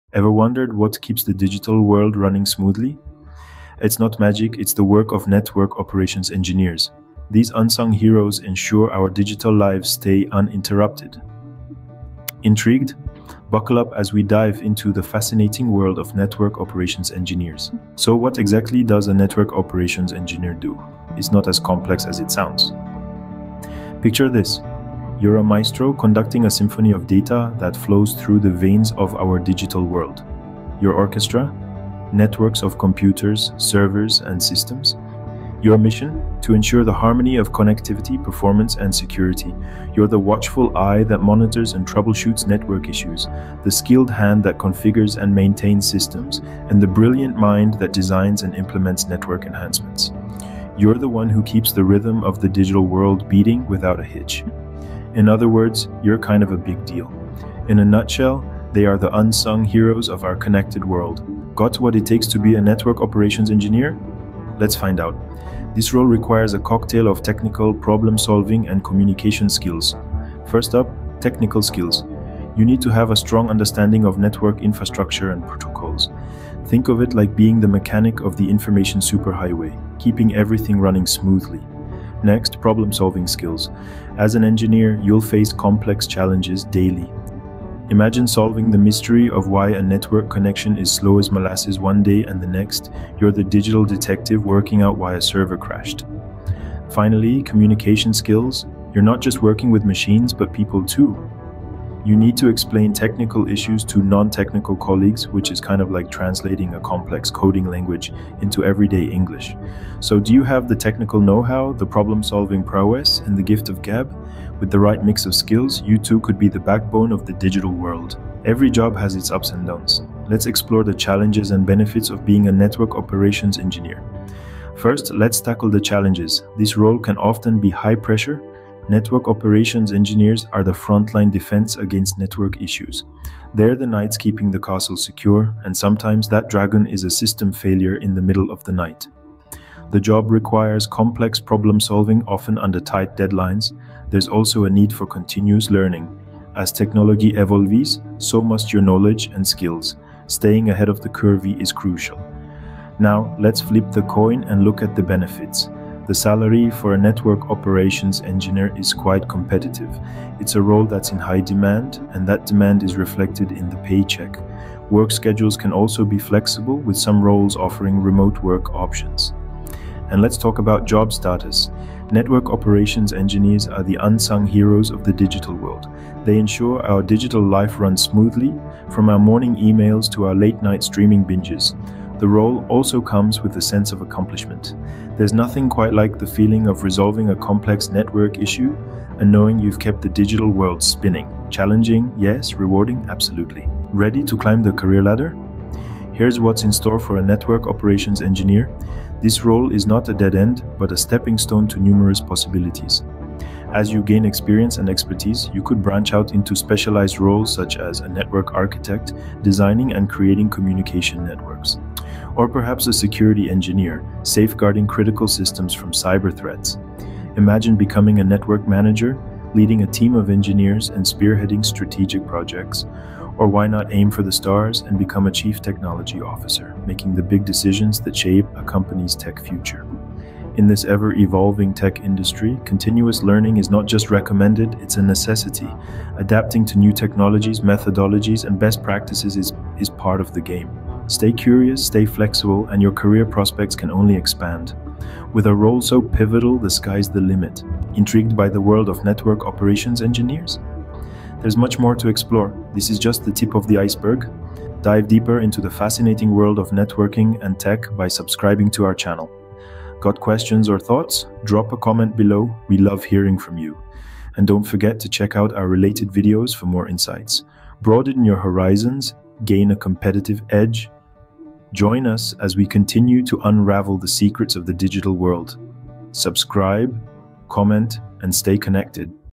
What is the role of a Network Operations Engineer ? | Career Guide - Job Description - Skills
In this video, we will walk you through a comprehensive guide to becoming a Network Operations Engineer. From understanding key job responsibilities to paving the way for career progression, we've got you covered! Whether you're a seasoned IT professional or just starting out in the industry, this video will provide you with valuable insights and tips to excel in this field. Let's dive in and explore the exciting world of Network Operations Engineering together!

🕰️ OUTLINE:

00:00:00 Introduction
00:00:17 The Role
00:01:01 The Skills
00:01:58 The Challenges & Benefits
00:03:13 The Opportunities
00:04:07 Conclusion

(⌒‿⌒)        ＼(≧▽≦)／        ヽ(o＾▽＾o)ノ        ☆ ～(‘▽^人)